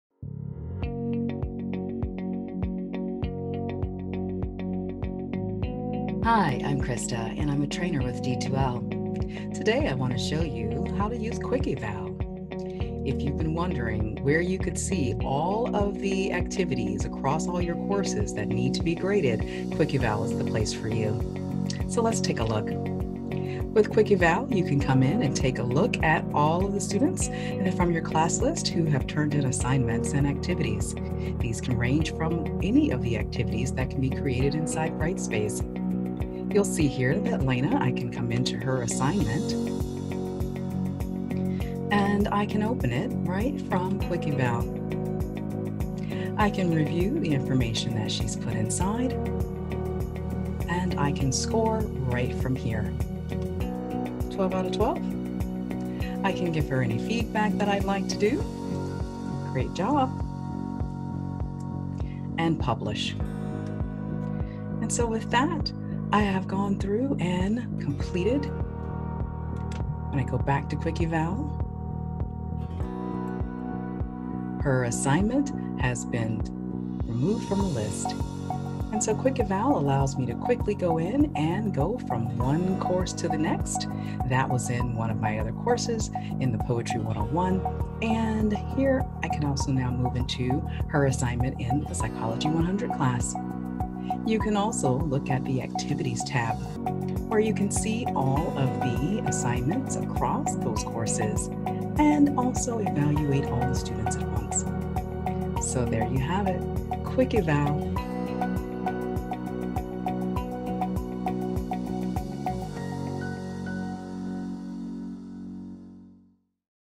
Find and prioritize student submissions quickly and easily with Brightspace
The Quick Eval tool in Brightspace lets you find and prioritize unevaluated student submissions quickly.